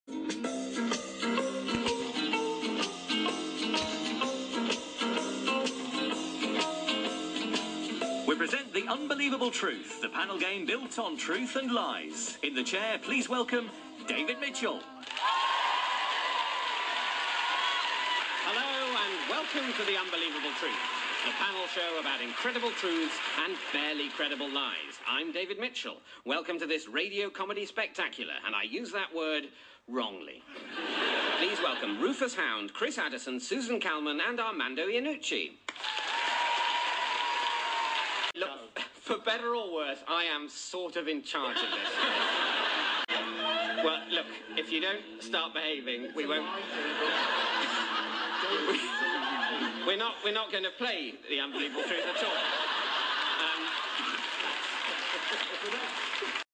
David Mitchell ~ The unbelievable truth experience
before and after the recording of TUT on 21. July 2010